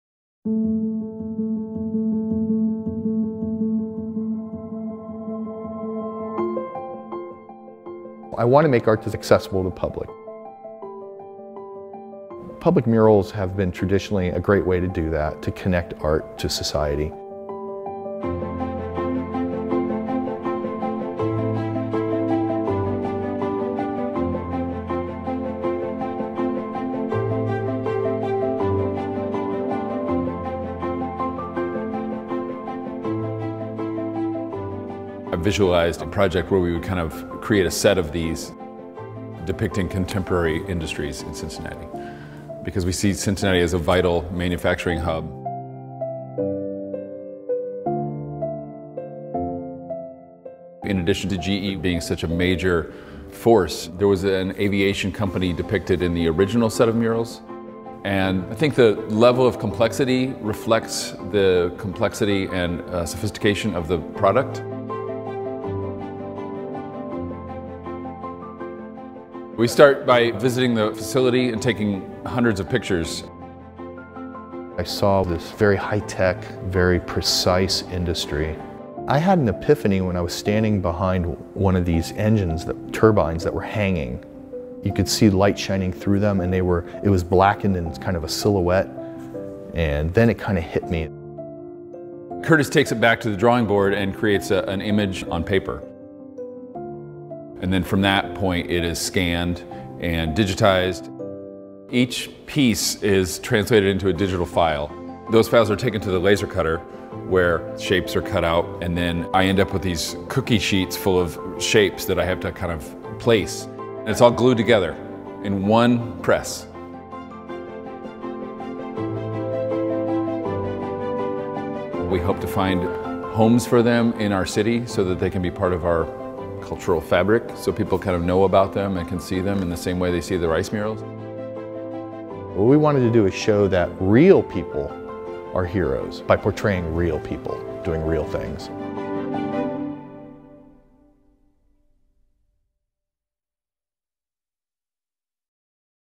Made in Cincinnati | GE Aviation Mosaic
Matt Lynch and Curtis Goldstein discuss the process of creating a mosaic series based on the 1930s Winhold Reiss murals. The project features GE Aviation as a prominent, local Cincinnati industrial company.